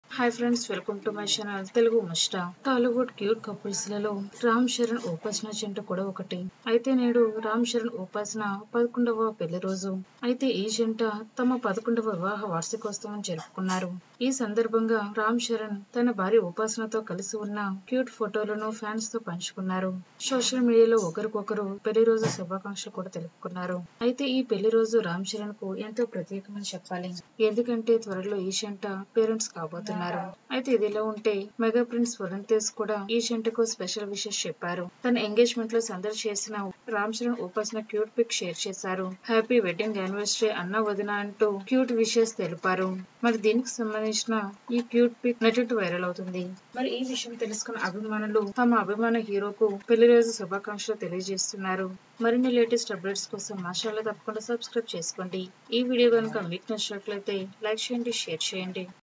రాంచరణ్ ఉపాసన పెళ్లి బంధానికి 11 ఏళ్ళు లేటెస్ట్ పిక్స్ వైరల్ Ramcharan Upasana 11th Wedding photos
రాంచరణ్ ఉపాసన పెళ్లి బంధానికి 11 ఏళ్ళు లేటెస్ట్ పిక్స్ వైరల్ Ramcharan Upasana 11th Wedding Celebrations  @telugumuchata